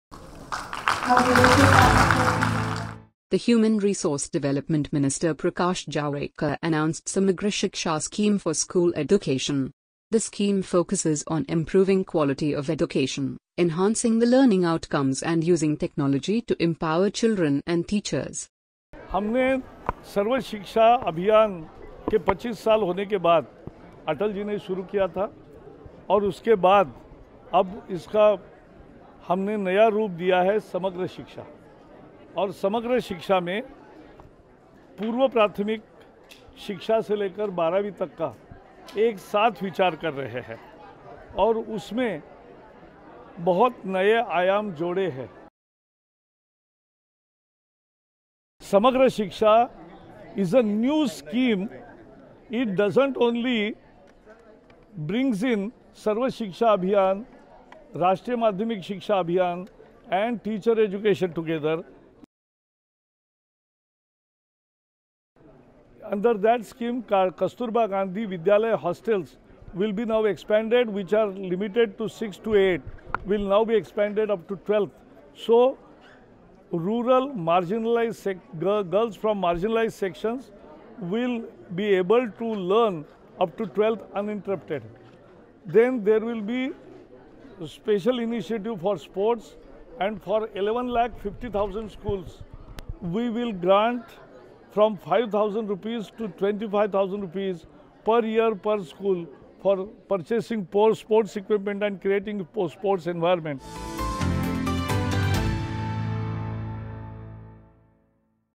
HRD Ministry Launches Samagra Shiksha Scheme
The Human Resource Development Minister Prakash Javadekar announced Samagra Shiksha scheme for school education. The scheme focuses on improving quality of education, enhancing the Learning outcomes and using technology to empower empower and teachers.